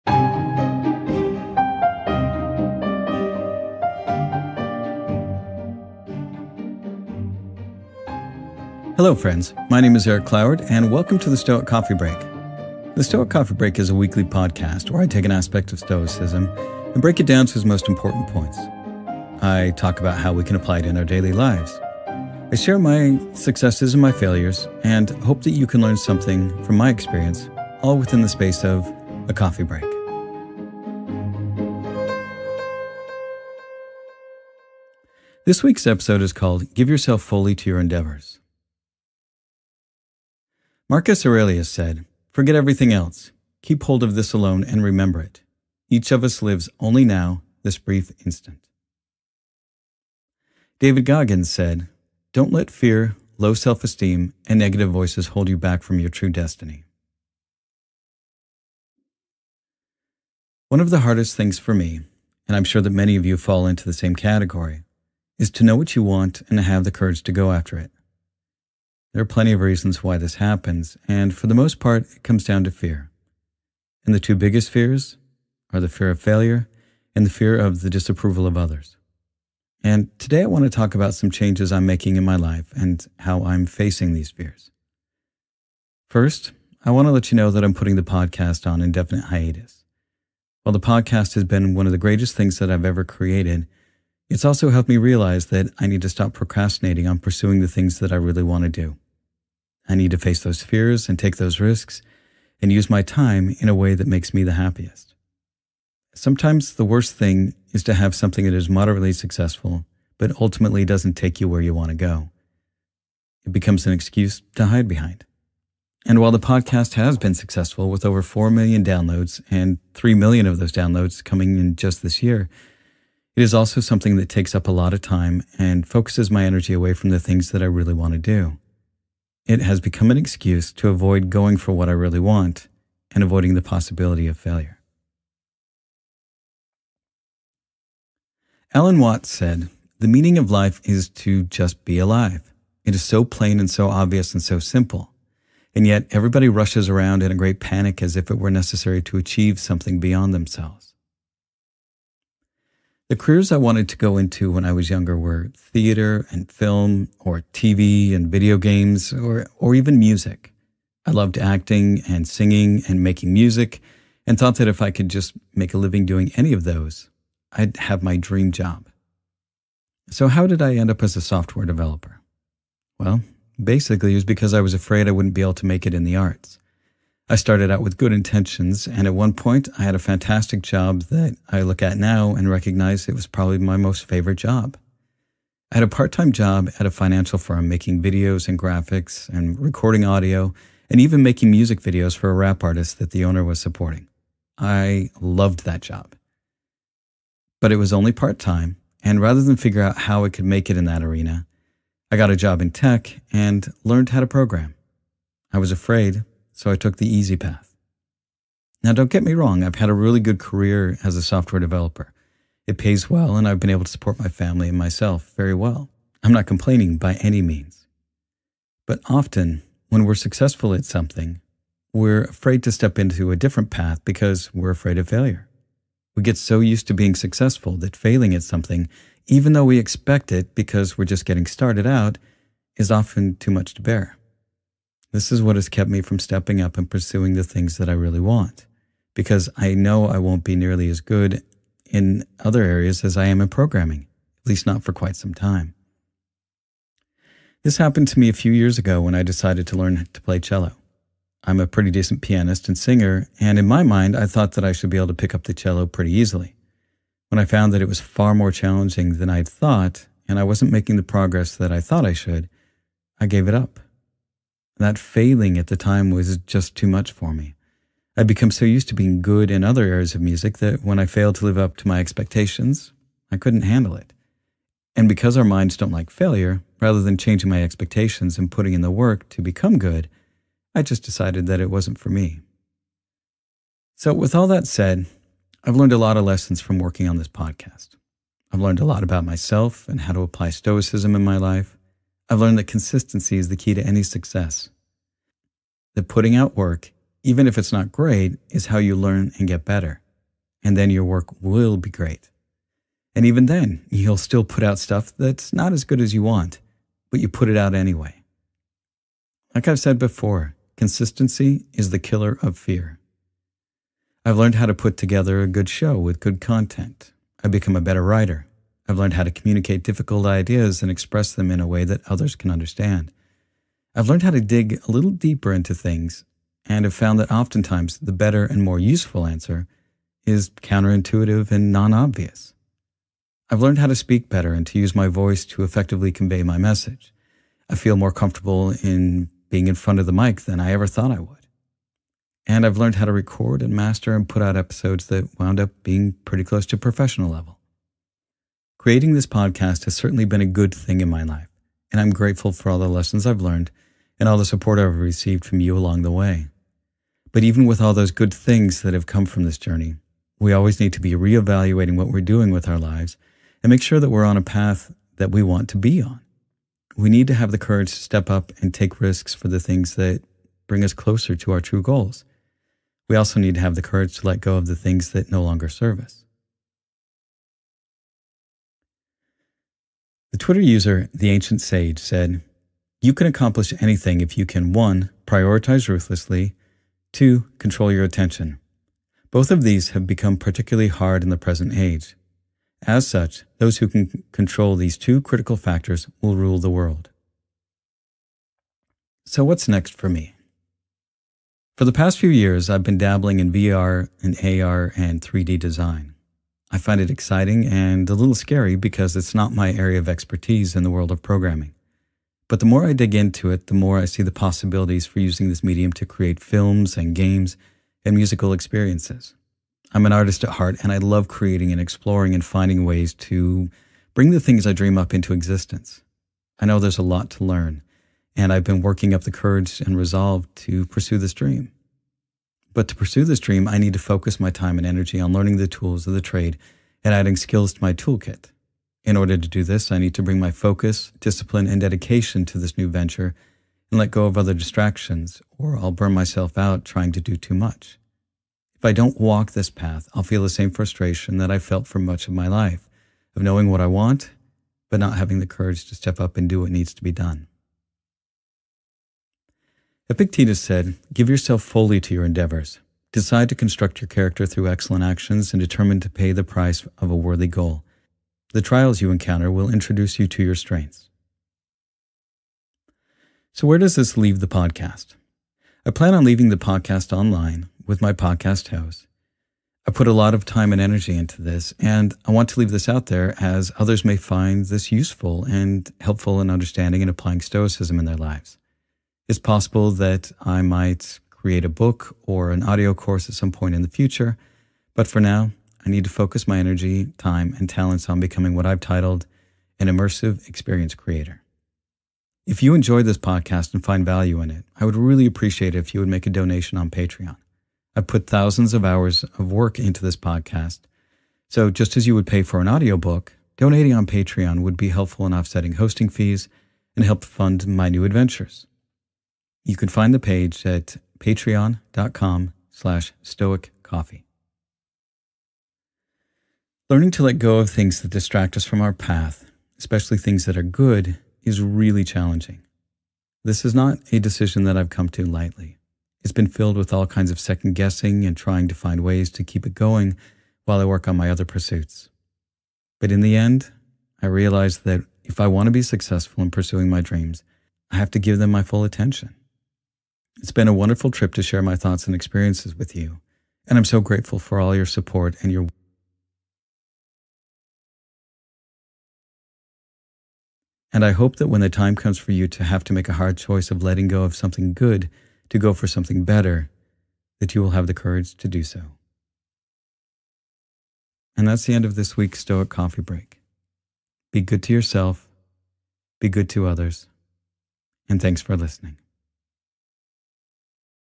216 - Give Yourself Fully To Your Endeavors
Forget everything else. Keep hold of this alone and remember it: Each of us lives only now, this brief instant.
—Marcus Aurelius

Don’t let fear, low self-esteem and the negative voices hold you back from your true destiny.
—David Goggins

One of the hardest things for me, and I’m sure that many of you fall into the same category, is to know what you want and have the courage to go after it. There are plenty of reason why this happens, and for the most part it comes down to fear, and the two biggest are fear of failure, and fear of disapproval of others. Today I want to talk about some changes I’m making in my life, and how I’m facing these fears.